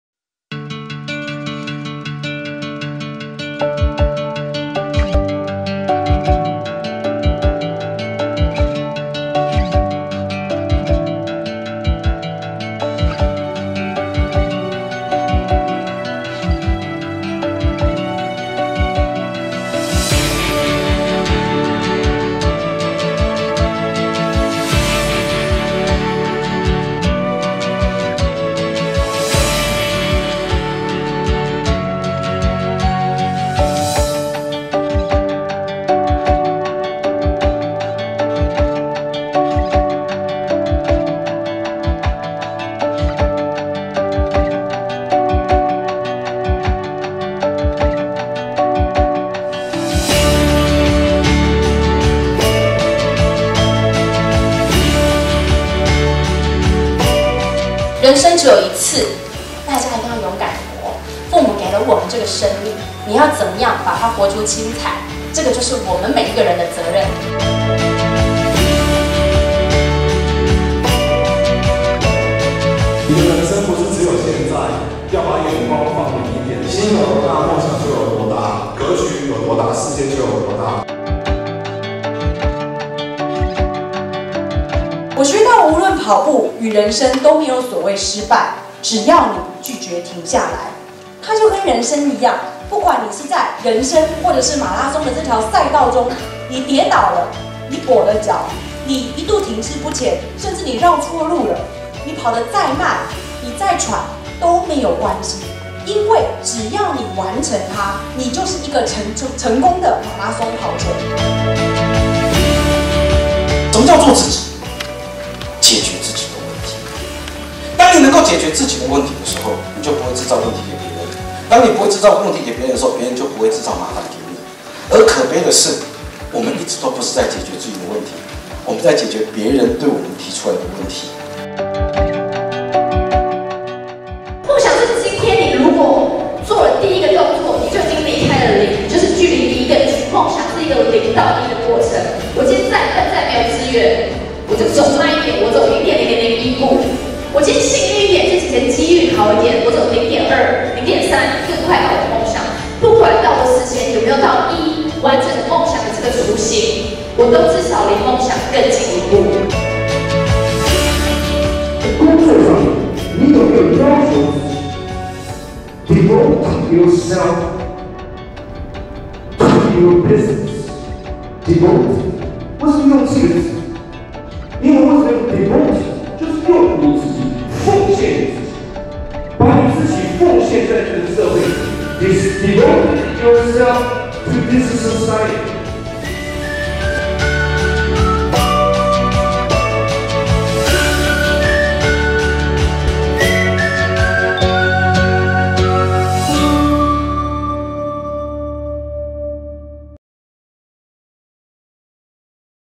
夢想無所畏！校園講堂【你的人生 自己設計】
所謂成功，是所有生活面向的加總都是你想要的！
成功的人生，需要被設計，而設計者，只能是你！
改變的鑰匙，其實握在你的手中。請無所畏懼的向你的夢想前進！